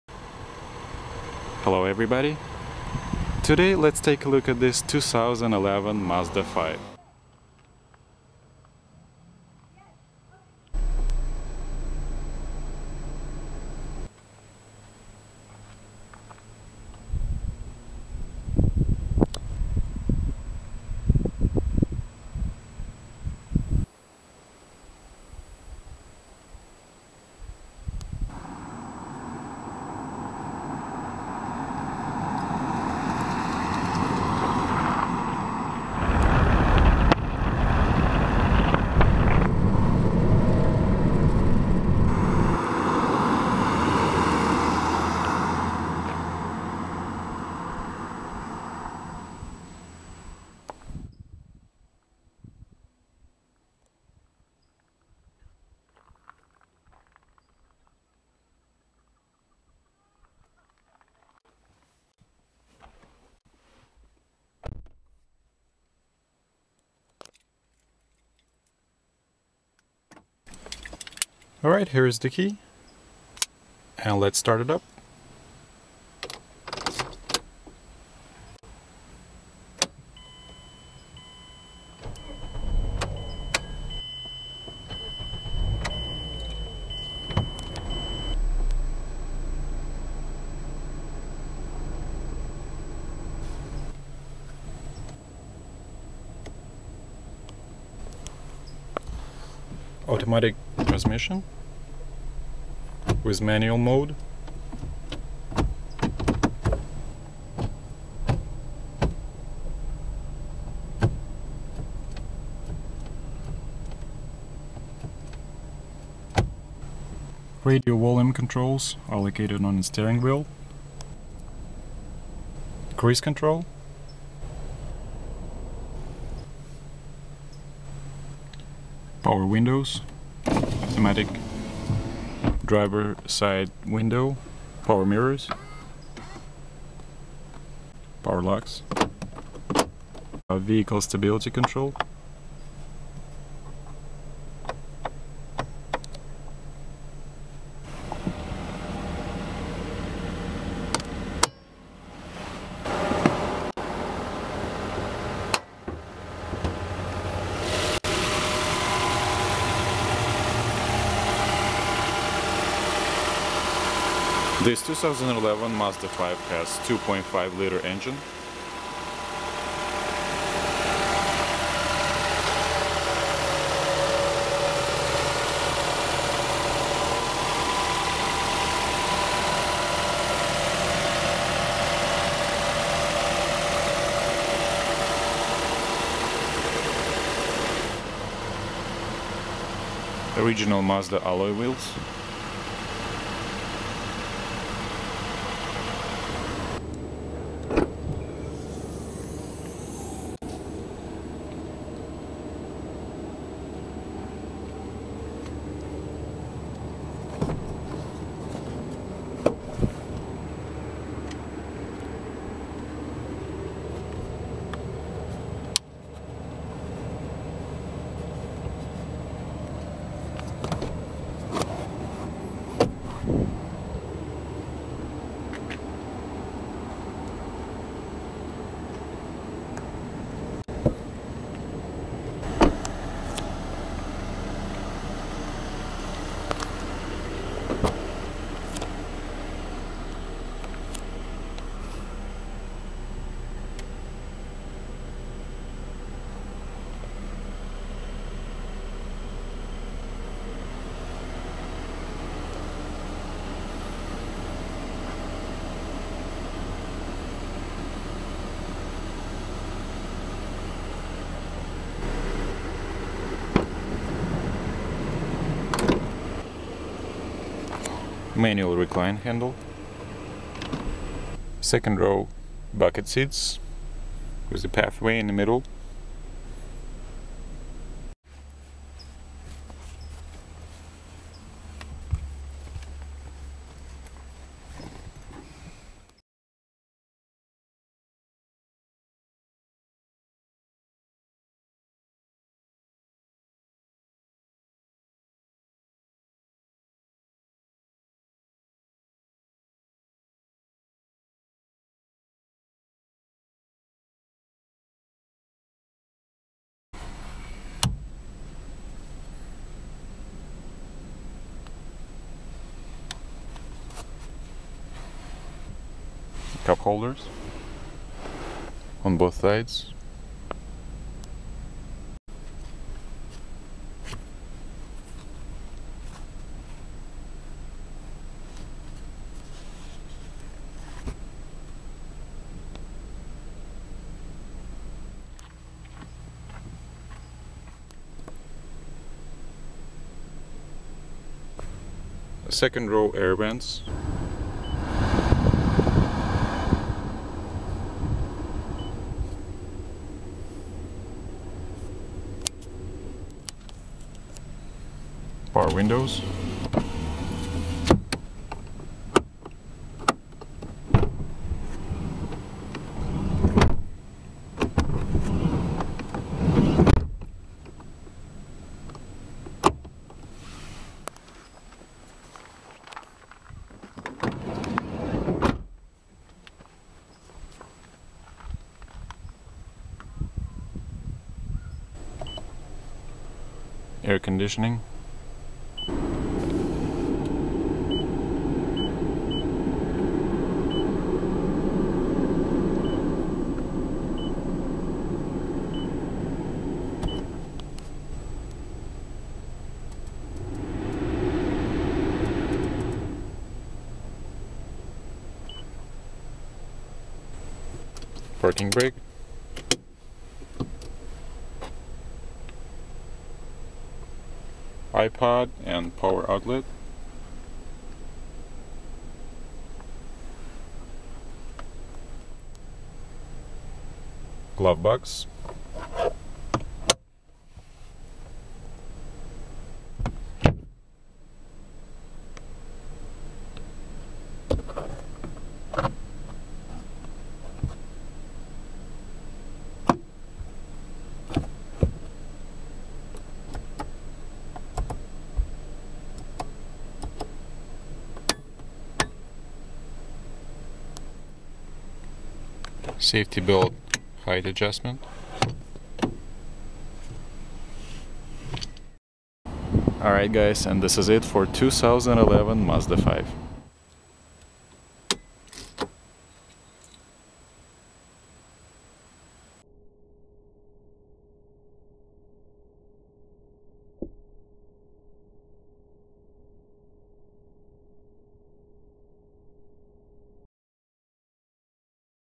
2012 MAZDA 5 OVERVIEW, Start up, Walk around tour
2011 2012 Mazda 5 Overview, Start up, Walk around tour, cold start, 157 hp (117 kW) 2.5-liter 4-cylinder engine AUTOMOTIVE REVIEW ON FACEBOOK